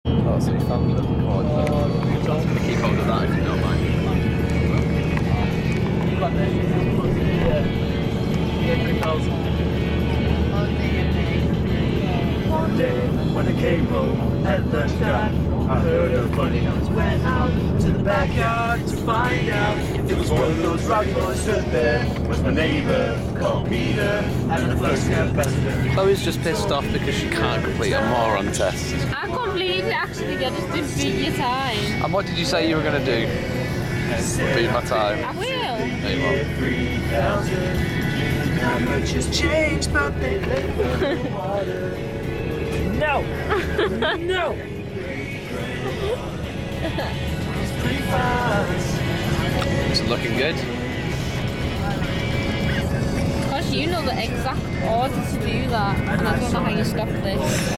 Austria 2012 - The Bus Journey
This is a fantastic summary of what happened on the bus journey back from skiing in Austria ;)